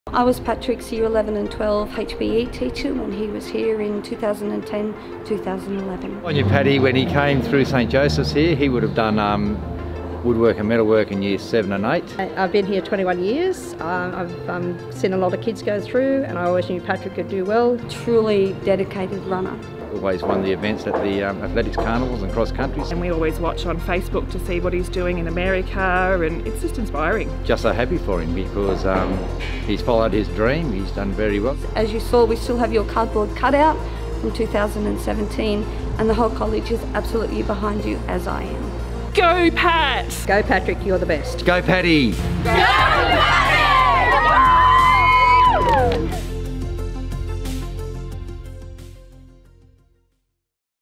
Past Student Patrick Tiernan competes in Tokyo Olympics
FROM TOOWOOMBA TO TOKYO!

We’d like to wish former St Joseph’s College student Patrick Tiernan the best of luck during the Tokyo Olympics! Pat will compete in the 5000m and 10,000m events.
Our entire community is incredibly proud of your achievements Pat, and can’t wait to watch you on the world stage.